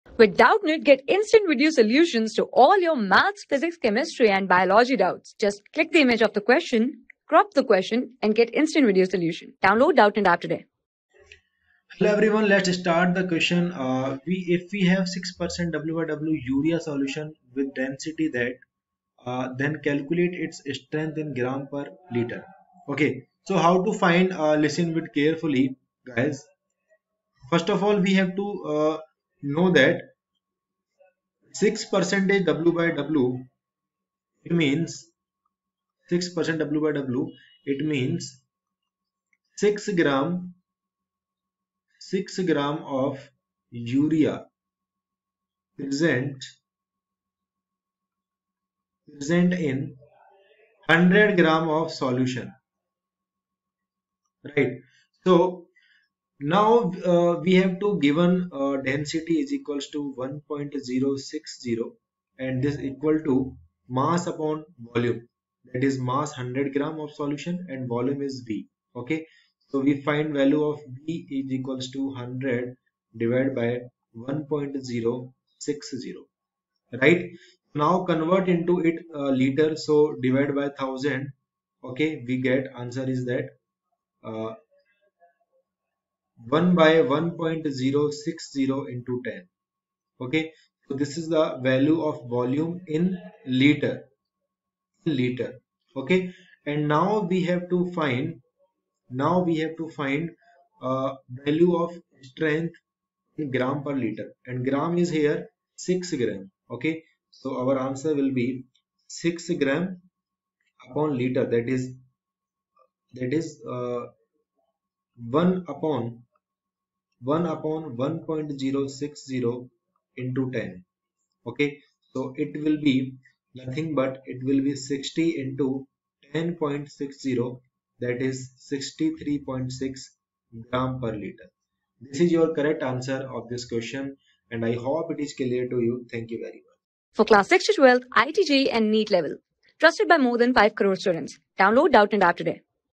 If we have 6% W//W urea solution with density 1.060g//mL, thencalculate its strength in g//L?
Class: 12
Subject: CHEMISTRY
Chapter: SOLUTIONS
Board:IIT JEE

👉 Have Any Query? Ask Us.